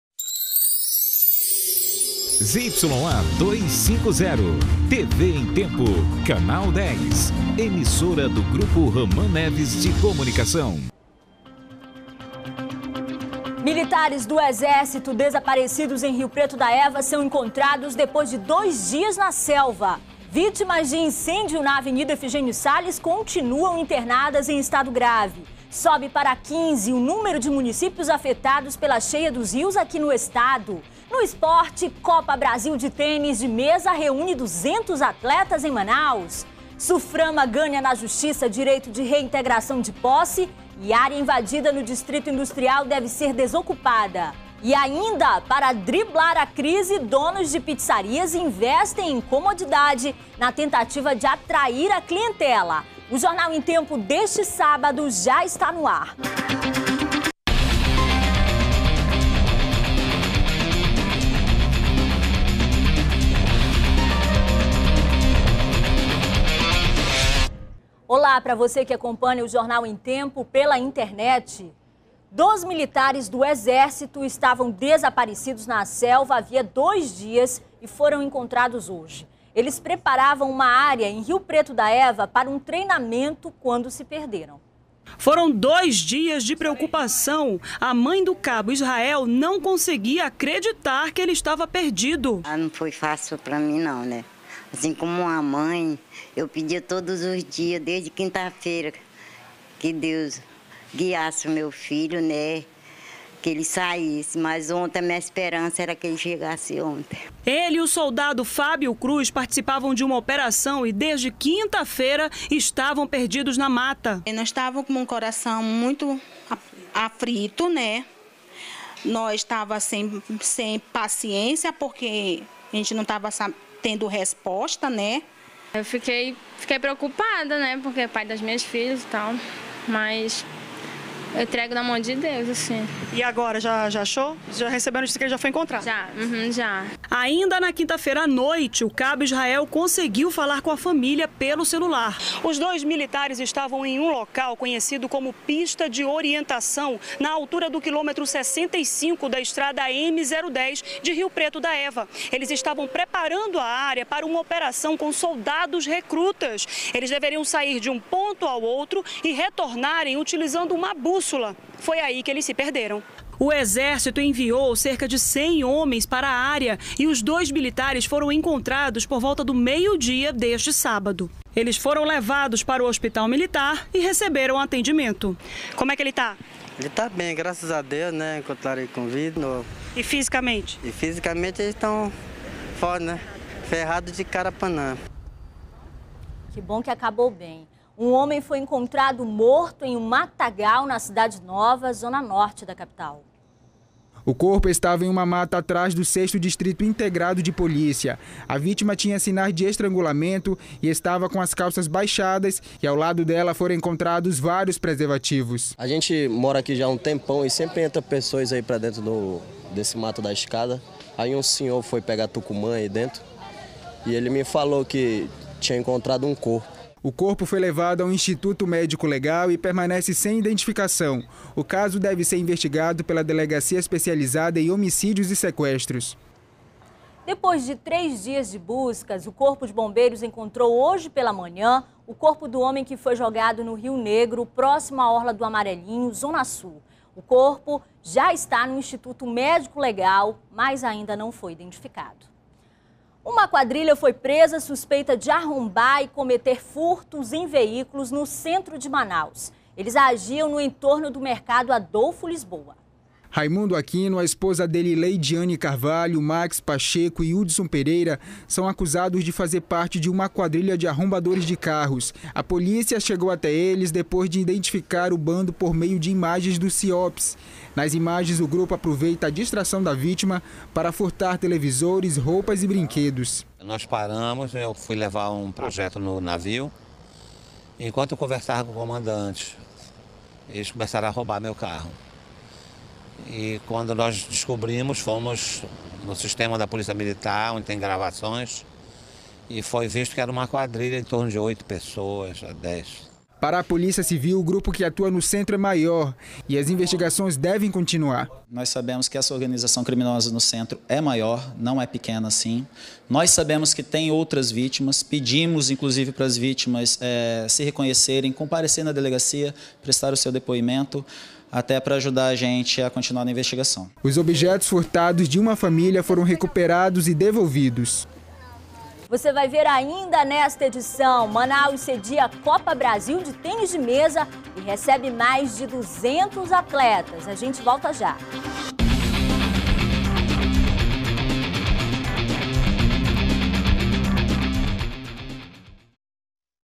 Militares do exército estavam desaparecidos na selva havia 02 dias e foram encontrados
01º BLOCO JORNAL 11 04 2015 - Militares do exército estavam desaparecidos na selva havia 02 dias e  foram encontrados